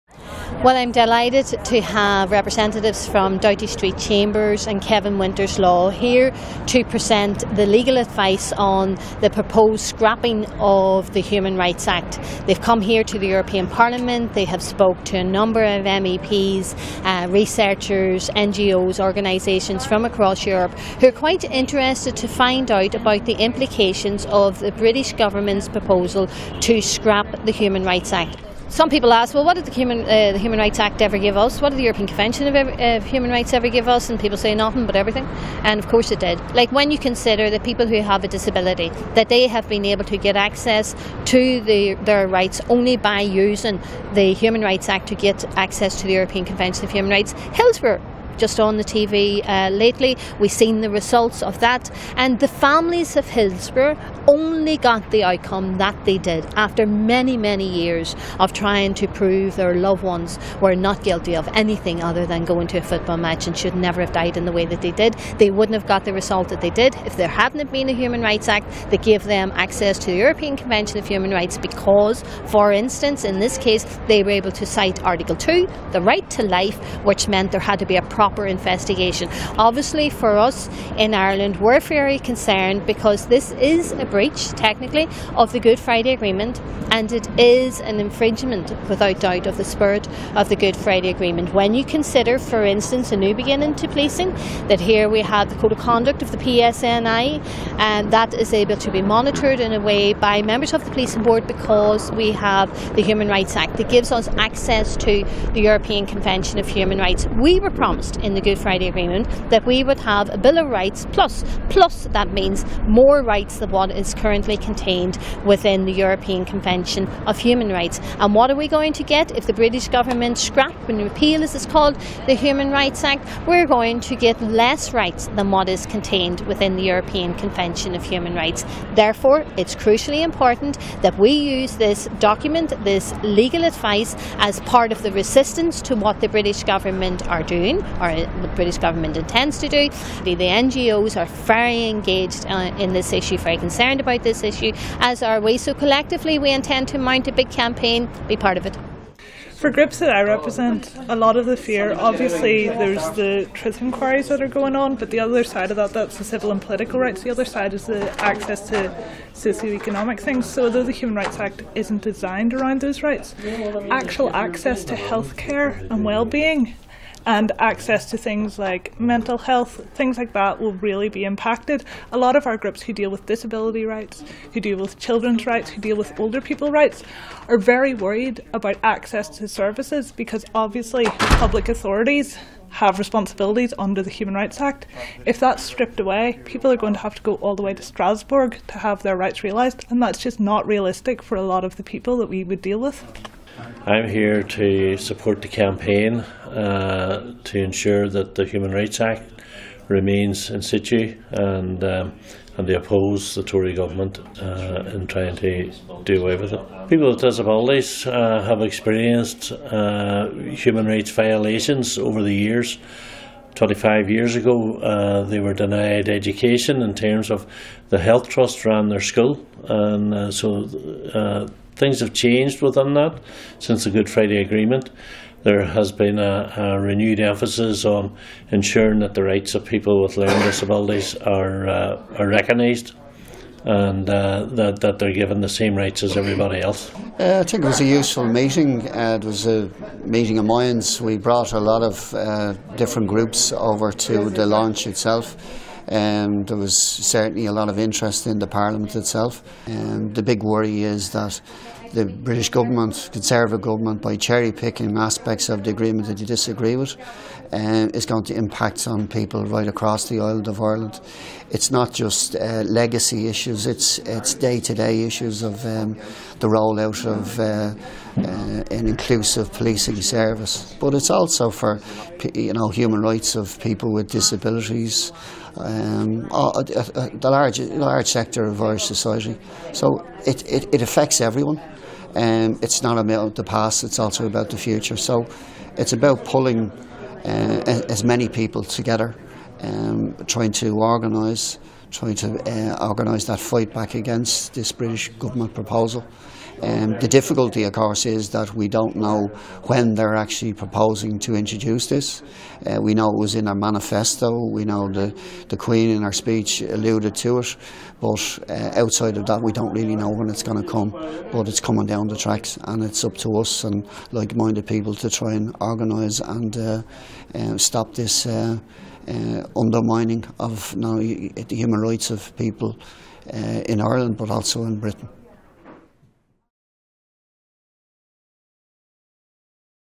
Protecting Human Rights - Defending the Human Rights Act
Sinn Féin MEPs in conjunction with the GUE-NGL political grouping in the European Parliament requested specifically commissioned legal advice on the British Governments proposal to repeal the 1998 Human Rights Act. On Wednesday 1st June this was launched in the European Parliament by Martina Anderson MEP, who was joined by delegates from Human Rights Consortium, Pat Finucane Centre, and Committee for the Administration of Justice as well as the authors of the legal opinion from Kevin Winters Law and Doughty St Chambers. The legal opinion confirmed Sinn Féin's belief that to replace the Human Rights Act would ‘perpetuate a degree of misinformation, disingenuous and muddying of legal principles’.